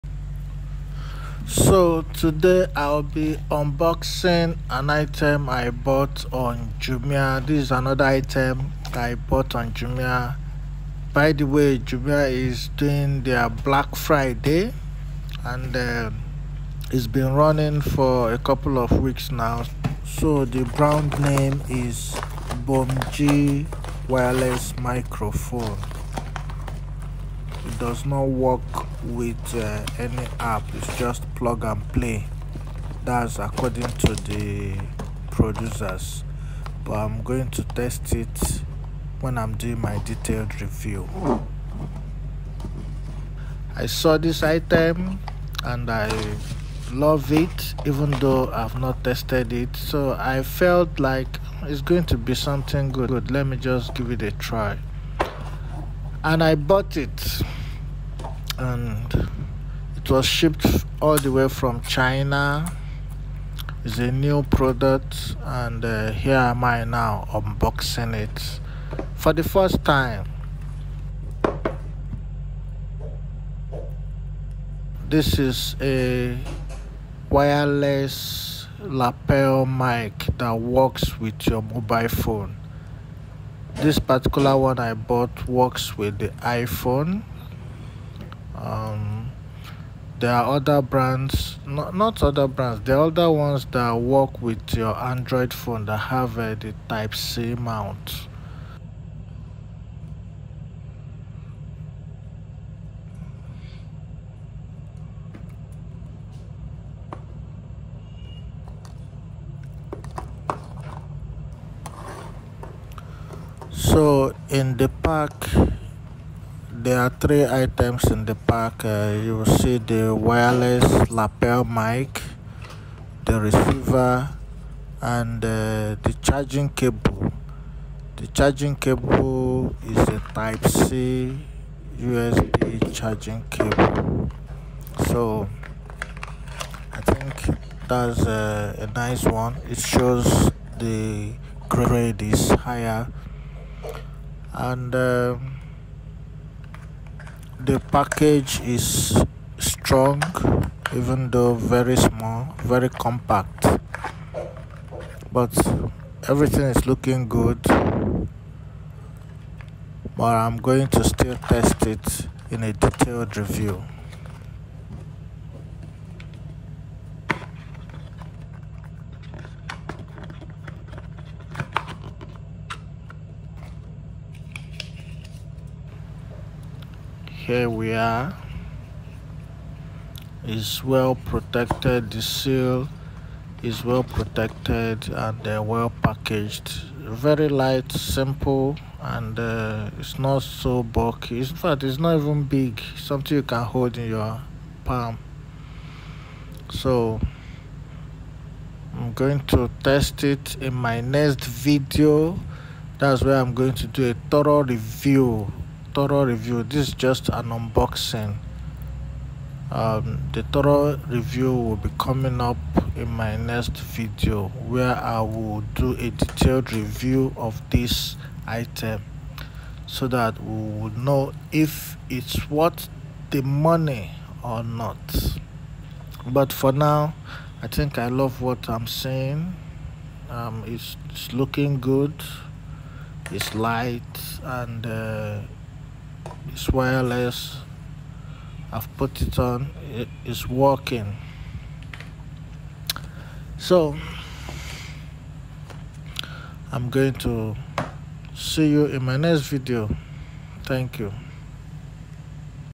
Unboxing A Jumia Black Friday Order - Bomge Wireless Lapel Microphone and Receiver for iPhone
In this video, I unboxed the Bomge wireless lapel microphone that works with an iPhone. I bought it on Jumia.com.ng during their black friday sales and christmas sales promo.